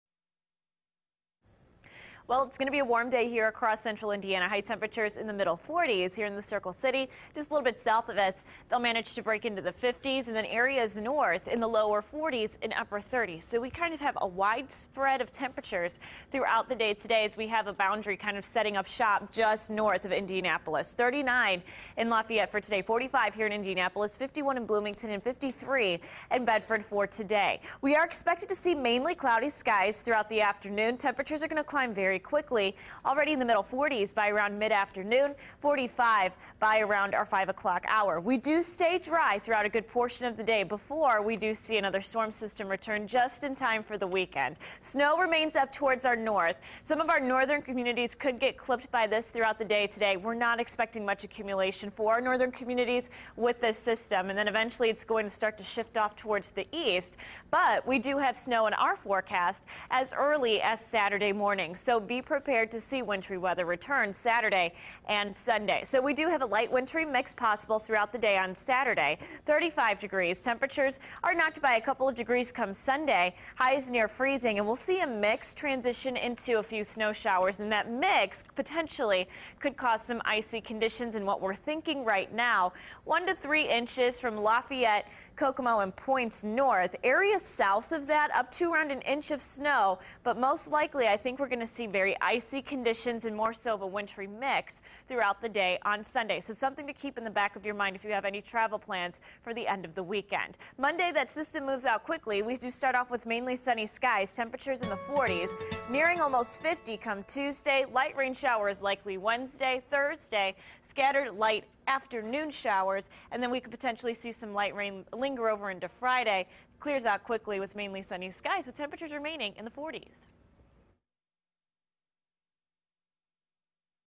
Friday's Forecast
Milder afternoon with highs in the mid 40s and cloudy skies.